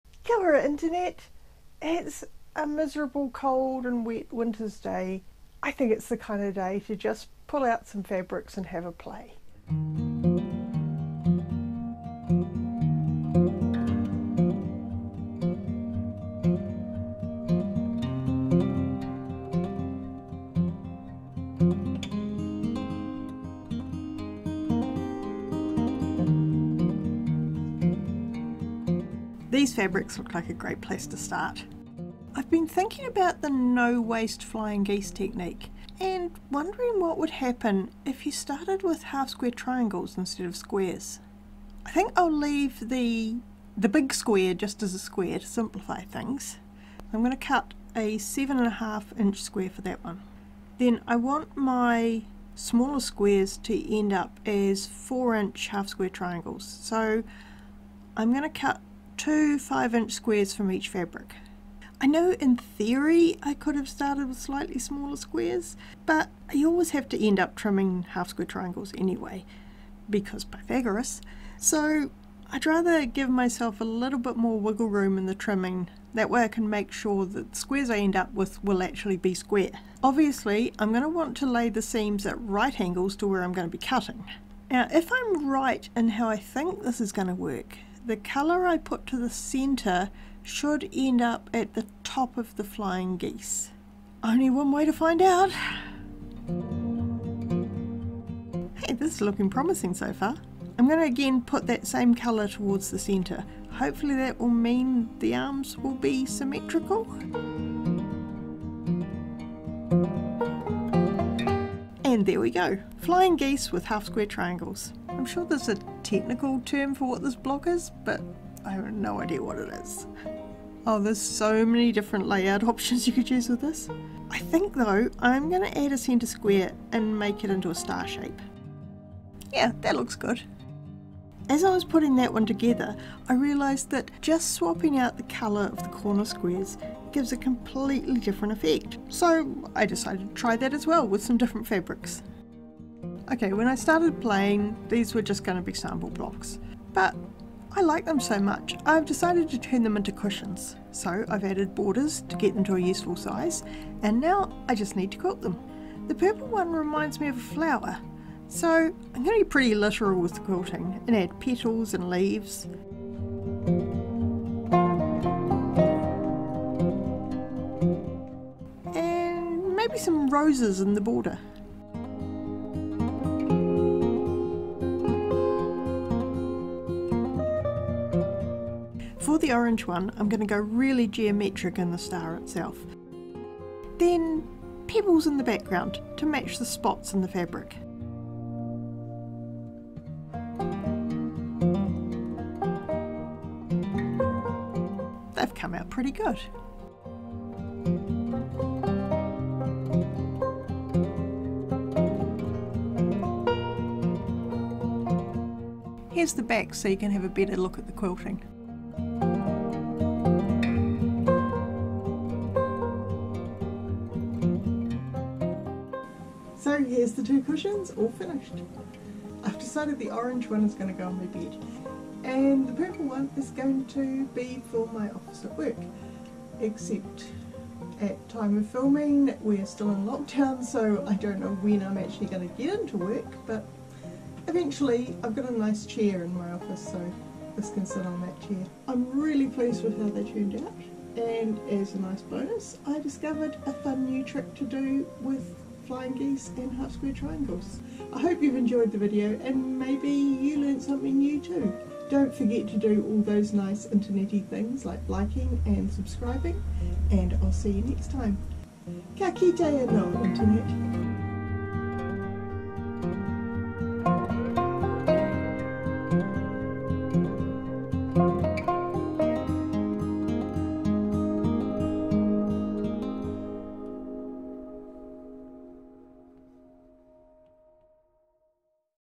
An experiment with flying geese
A little experiment on a cold and grey day turned into two bright and beautiful cushions.